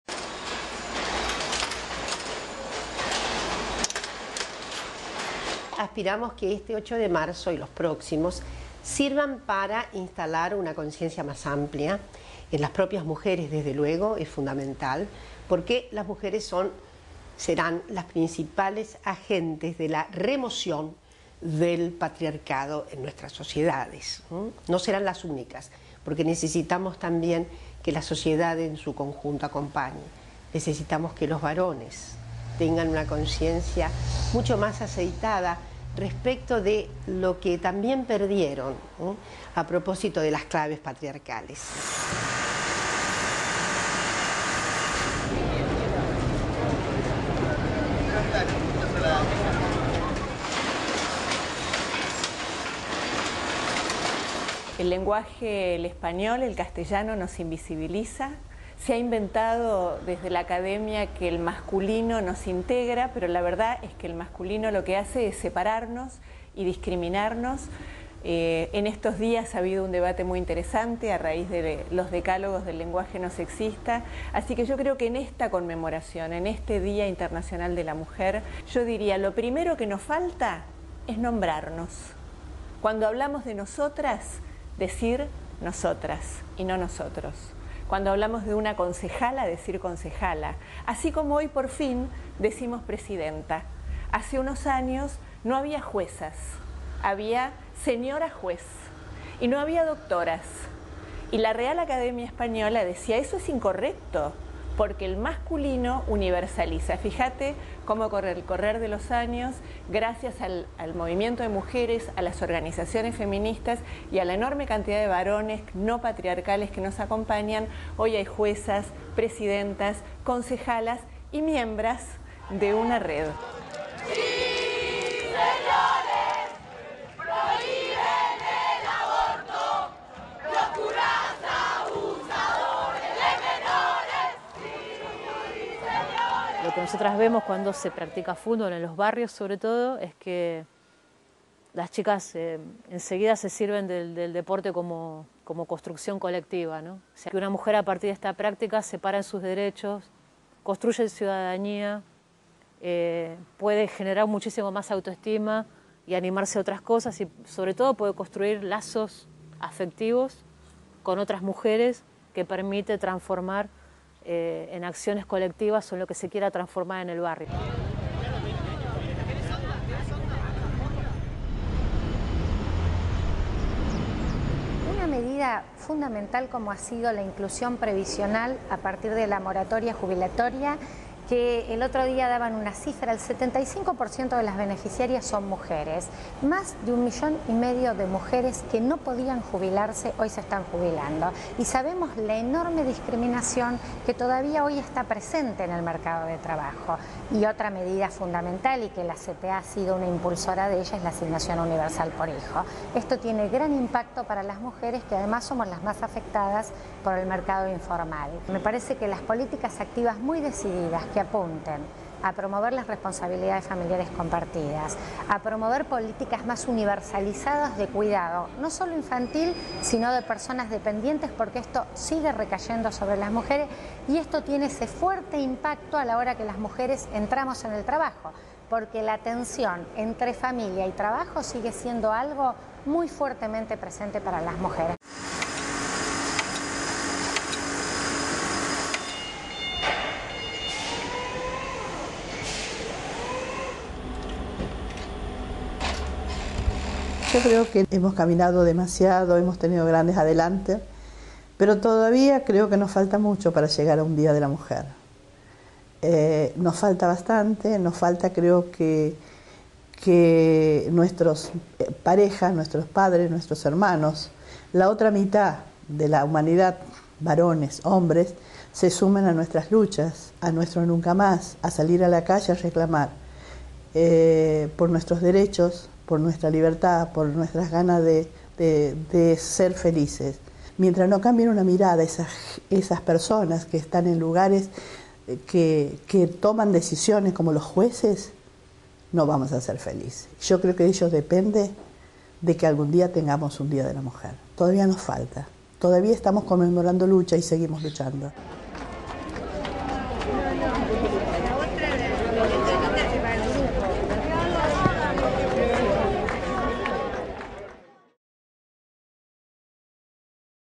8 de marzo: Día Internacional de la Mujer
Referentes de distintos ámbitos reflexionan acerca del Día Internacional de la Mujer, dan cuenta de los avances y los temas pendientes en materia de igualdad de género y destacan que sigue siendo un día de lucha.
La doctora en ciencias humanas e investigadora del Conicet, Dora Barrancos sostuvo que las mujeres son las principales agentes para la remoción del patriarcado.
Con respecto al lenguaje, la periodista e integrante de la Red de Periodistas por una Comunicación no Sexista (REDPAR), Liliana Hendel dijo que "el lenguaje nos invisibiliza, lo primero que nos falta es nombrarnos".
Para la directora técnica nacional de fútbol y ex jugadora de All Boys, Mónica Santino "una mujer que practica fútbol se para en sus derechos, construye ciudadanía y genera autoestima".
"Más de un millón y medio de mujeres que no podían jubilarse hoy lo están haciendo" sostuvo Estela Díaz del Centro de Estudios Mujeres y Trabajo  de la Central de Trabajadores Argentinos (CTA).
Nelly Borquez, coordinadora de la casa de la mujer Rosa Chazarreta de La Matanza, provincia de Buenos Aires remarcó que "nos falta que nuestras parejas, los varones, se sumen a nuestro nunca más para poder ser felices".